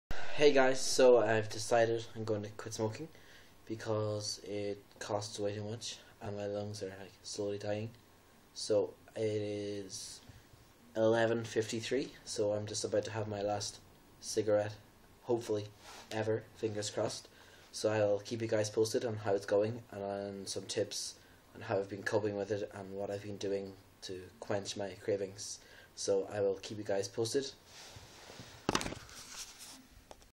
I'm Quitting smoking.
I'm quitting smoking, My last cigarette. Gonna keep a Vlog of my journey and challenges and how I overcome them :)